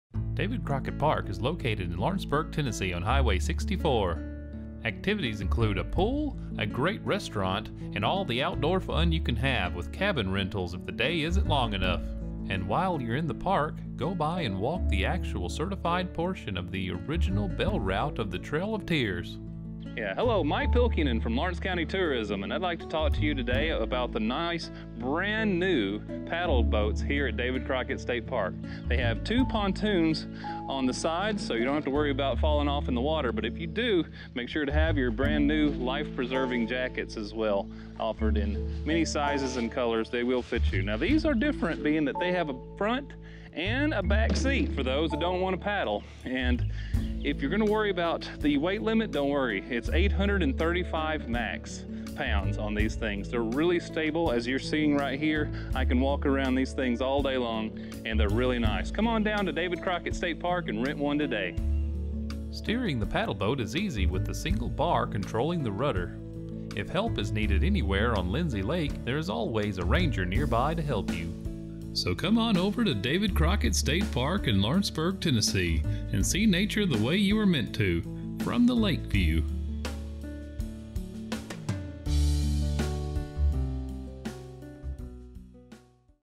david crockett park gets new paddle boats
in lawrenceburg tennessee david crockett park gets new paddle boats for the summer. 835lbs weight limit. $5 per person per hour.